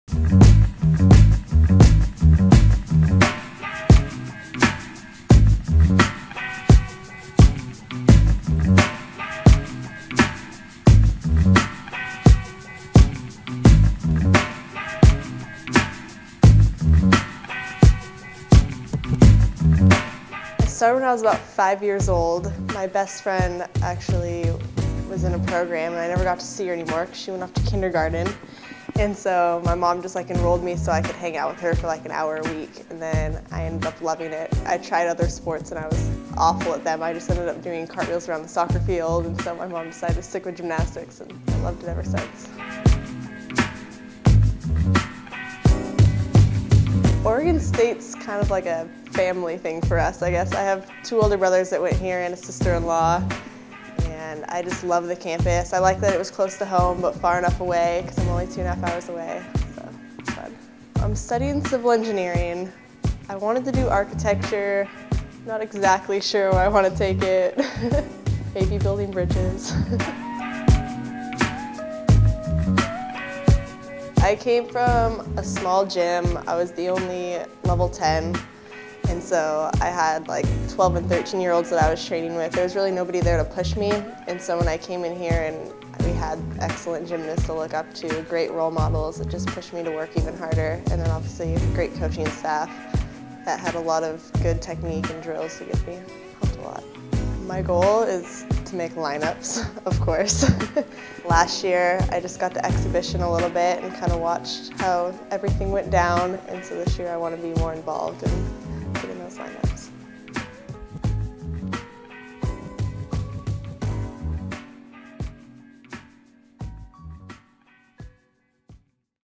Meet the Beavers: Katelyn Ohlrich
Meet Katelyn Ohlrich, a sophomore All-Around competitor on the Oregon State Gymnastics team from Bend, Oregon in this edition of "Meet the Beavers."